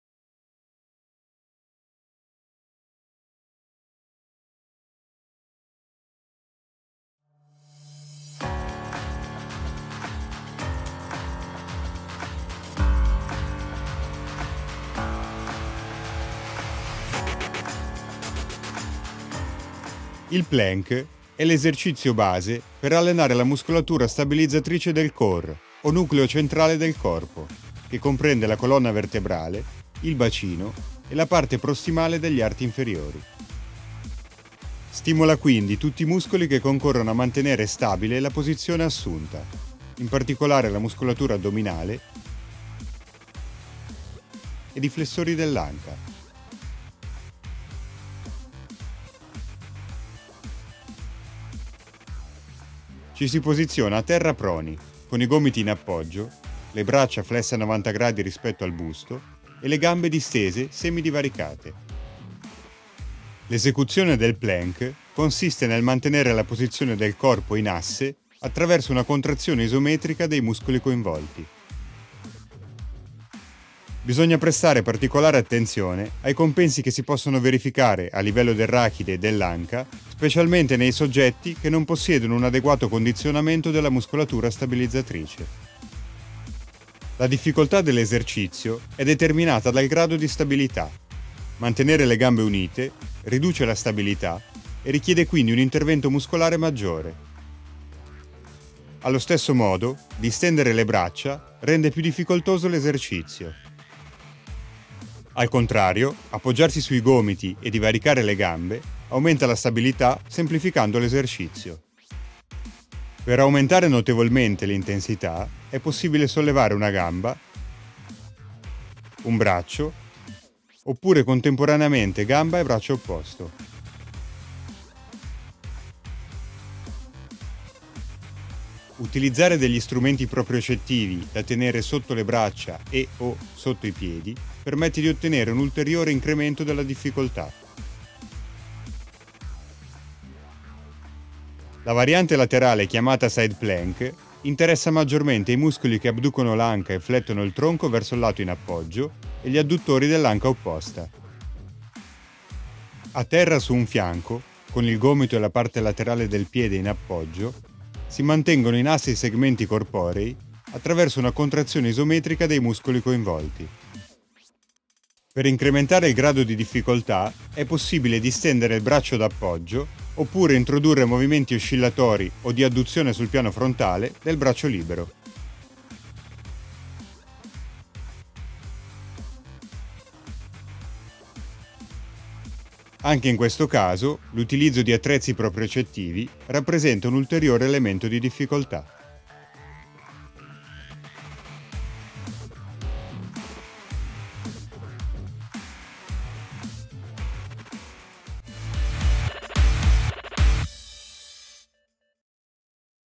PLANK
Plank: esecuzione, muscoli coinvolti, varianti, indicazioni e controindicazioni.
Definizioni, schede tecniche, video esercizi, articoli tecnico-scientifici.

Atleta: Federico Zaminato